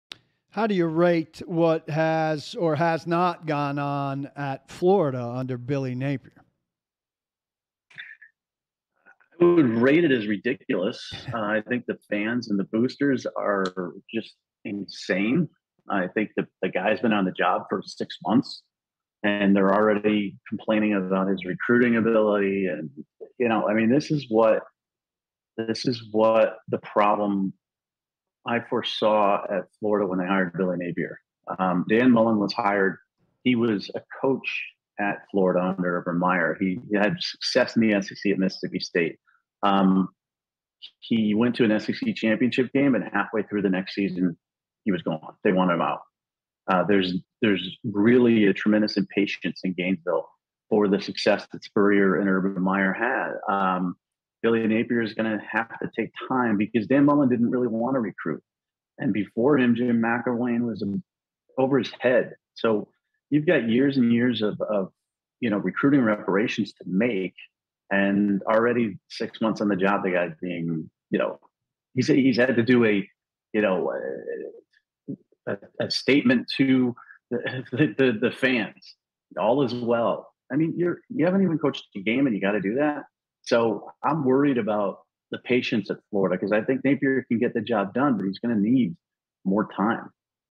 Mike Farrell: Florida Fans & Boosters Are Insane
Mike Farrell discusses Billy Napier in Florida and what to expect from fans and boosters.